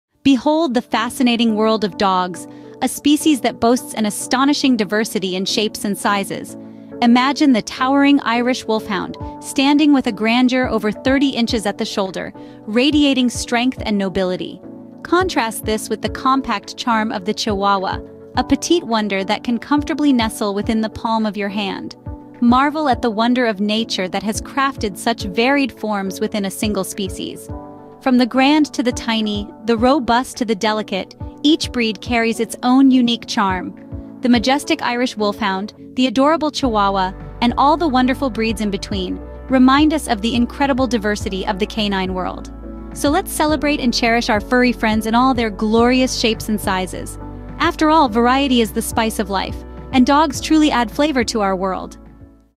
Diversity in Dogs: From Giants to Tiny Paws 53 second
Dogs, the epitome of diversity in the animal kingdom, present an astonishing array of shapes and sizes. Ranging from the majestic stature of the towering Irish Wolfhound, standing over 30 inches tall at the shoulder, to the adorable diminutiveness of the Chihuahua, small enough to nestle in the palm of your hand, the breadth of variation within the species is truly remarkable. Each breed encapsulates unique physical characteristics, contributing to the rich tapestry of canine diversity. From giants to miniatures, dogs captivate our hearts with their distinct appearances and personalities, showcasing the endless wonders of nature's design.